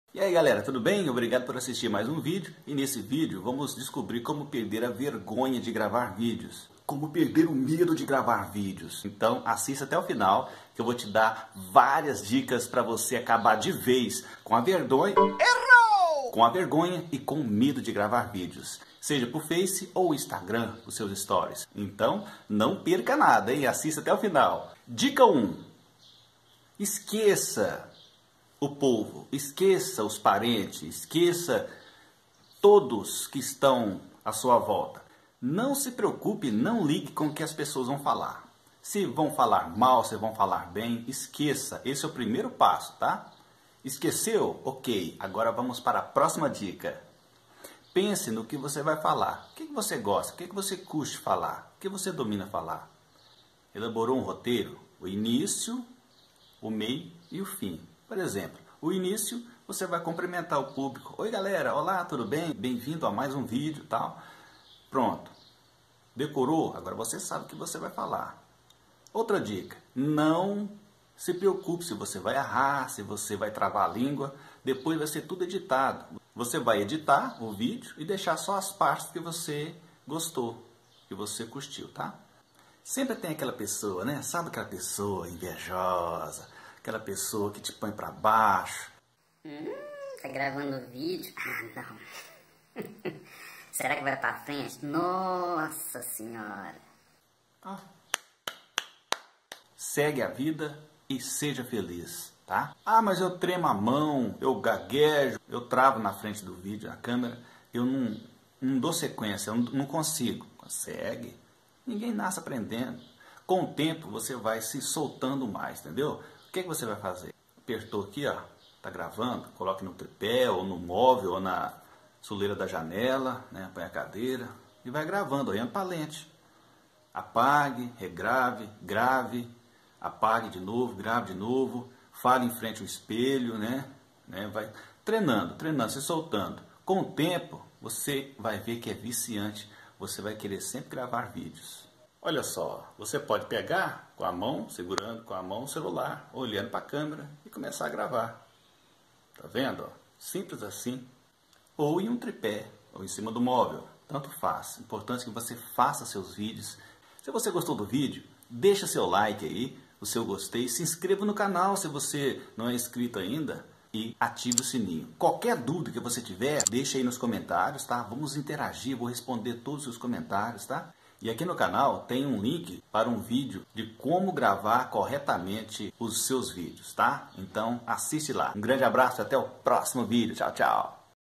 Como Perder a Vergonha de Gravar Vídeos método aprovado
Saiba como perder a vergonha de gravar videos e fazer sucesso, com super dicas para te ajudar a superar o medo de gravar videos para o youtube, Facebook, Instagram etc.
⚫ Esqueça as pessoas e o que elas vão falar..
⚫ Escolha um assunto para o seu video e elabore um roteiro (inicio, meio e fim)
⚫ Não se preocupe com erros de gravação, pois isso sera cortado na edição.
⚫ Treine muito gravando e regravando...
Gravar video hoje em dia é super importante para quem quer trabalhar na internet divulgando, expressando ideias e muito mais...
Gostou do video? Então compartilhe 😀: 📹 Como Perder a Vergonha de Gravar Vídeos I DICAS para superar!